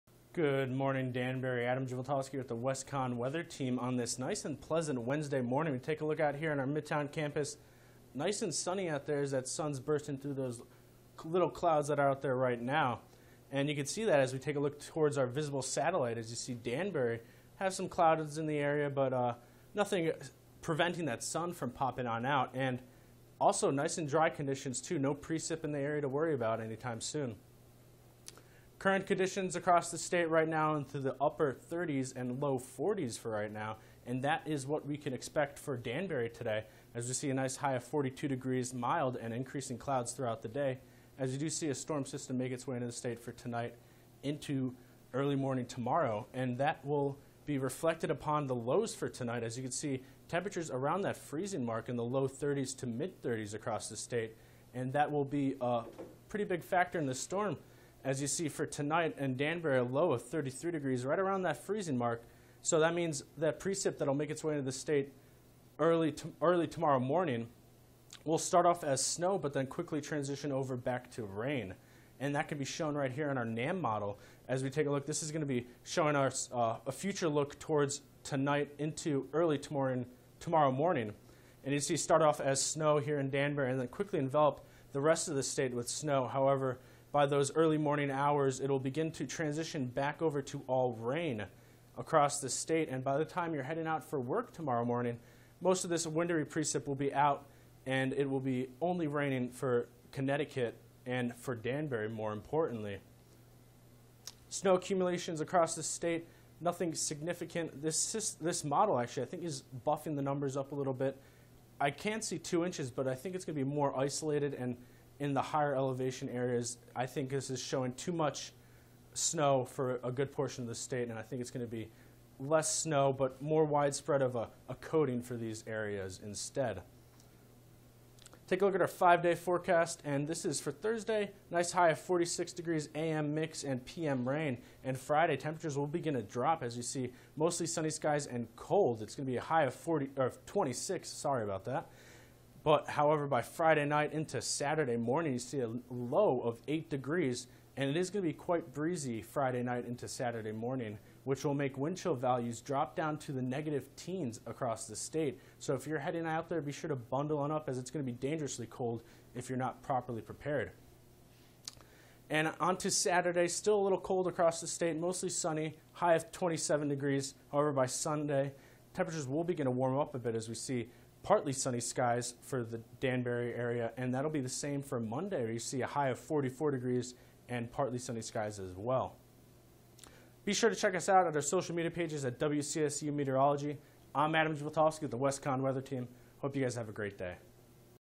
Adam Dziewaltowski, Wednesday, February 12th, 2020
The sun has returned after a few days of rain, however we have a system making its way toward us for late tonight. What can we expect to see tonight? How will your morning commute be impacted tomorrow?